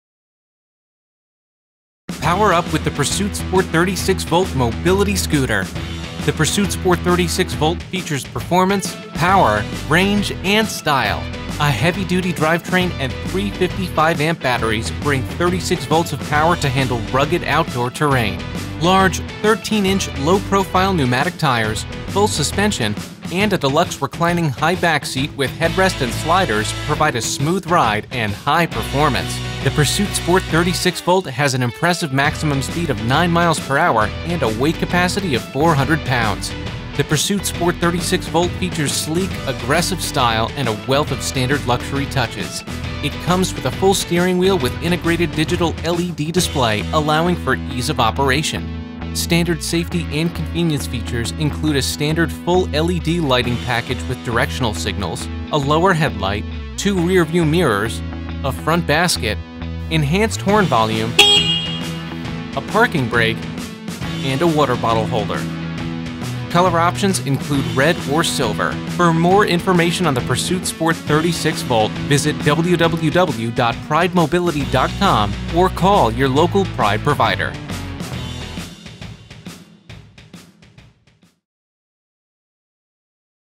Pursuit® Sport 36 Volt Mobility Scooter Overview
Pride FDA Class II Medical Devices are designed to aid individuals with mobility impairments.

Learn more about the Pursuit® Sport 36 Volt Mobility Scooter from Pride Mobility Products®. With features like 36 volts of power, full suspension, and a hydraulic sealed brake system, the Pursuit® Sport 36 Volt is the ultimate outdoor scooter.